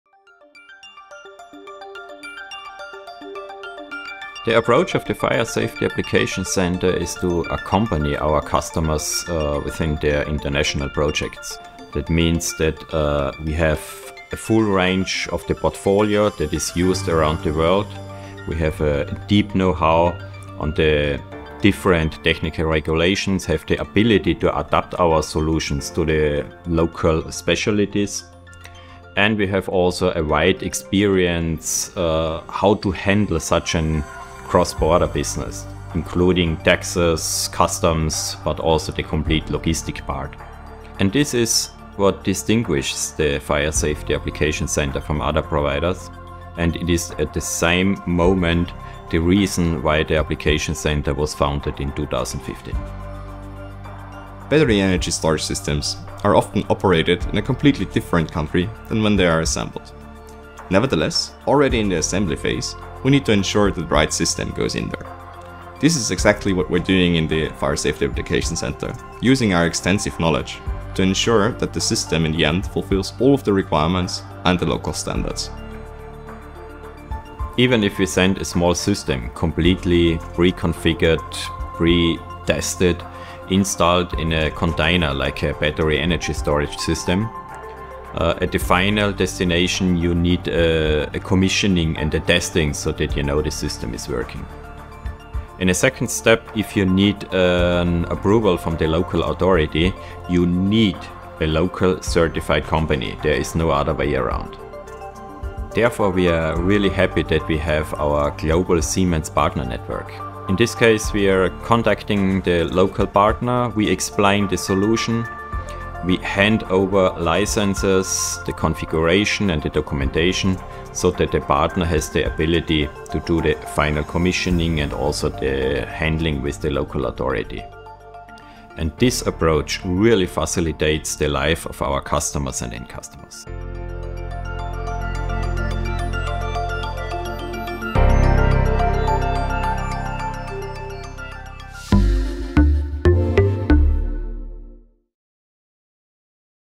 Siemens Fire Safety Application Center – Customized Fire safety solutions
Discover the Siemens Fire Safety Application Center, your trusted partner for international fire safety projects like lithium-ion battery energy storage. With a profound understanding of regulations and the ability to tailor solutions to customer needs, we ensure compliance and efficiency.

Learn how we streamline cross-border fire safety projects, from system assembly to commissioning, testing, and handling with local authorities.